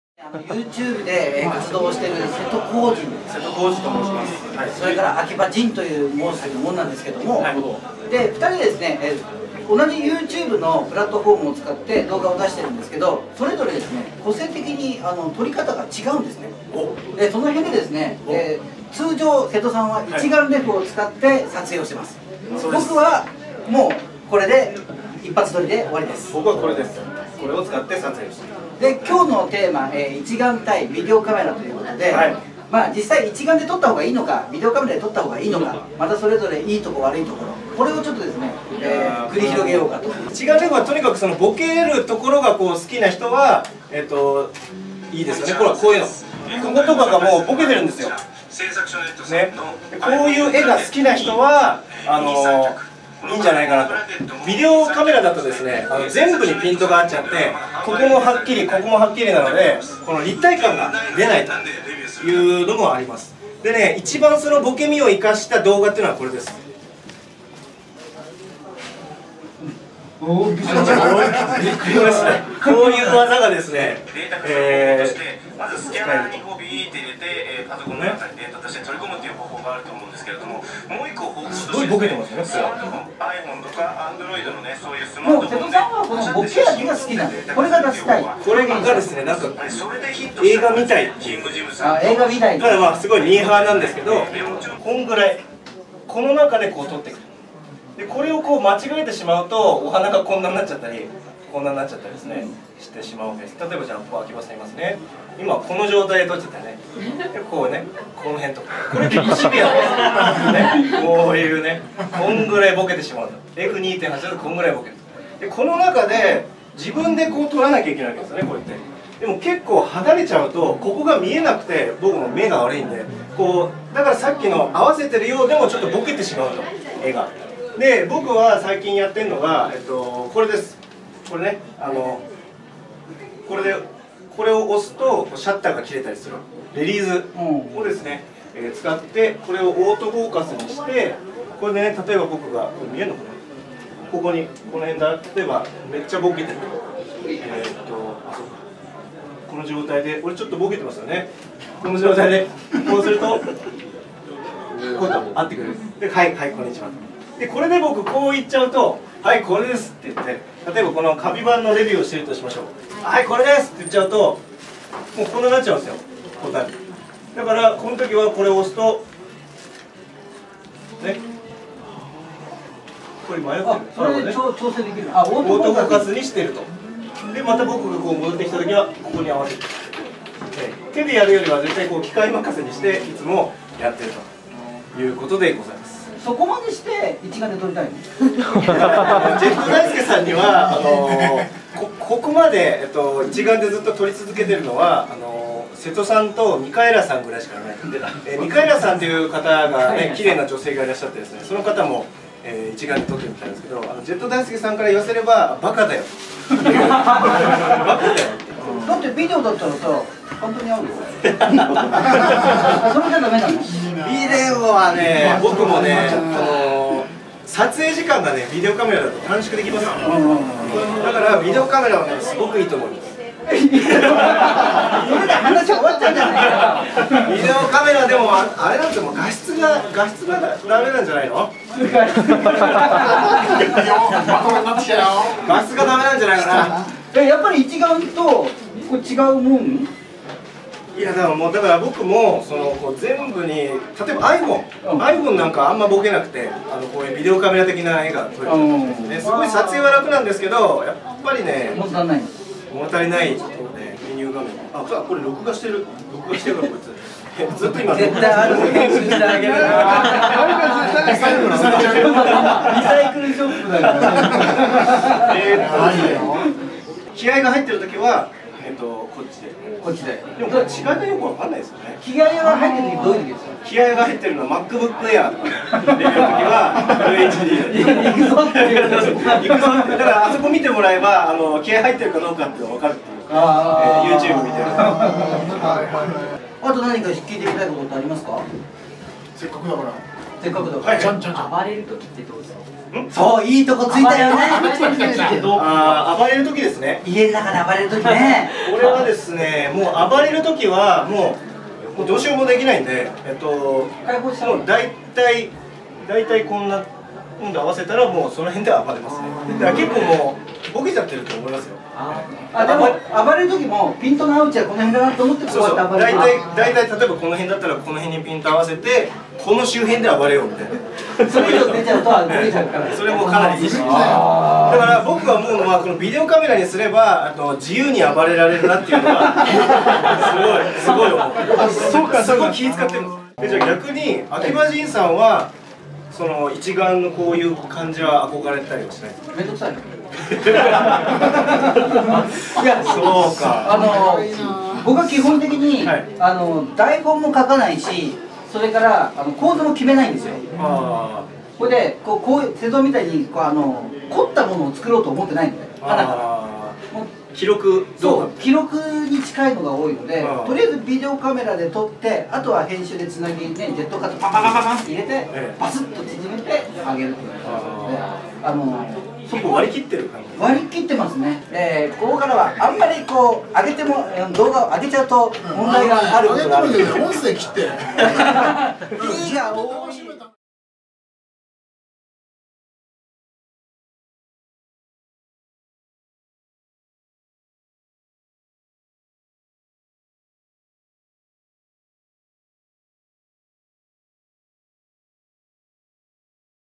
瀬戸弘司 × 秋葉仁 ARTALK3 花キューピット東京トークステージ
デザインフェスタギャラリー原宿にて行われたARTALK3の1Fブースにて「 花キューピット東京 トークステージ 」presented by  花キューピットTOKYOにて行われた秋葉仁さんと瀬戸弘司さんによる「ビデオブロガートークバトル」です。後半はオフレコトークですのでご了承下さいませ。 .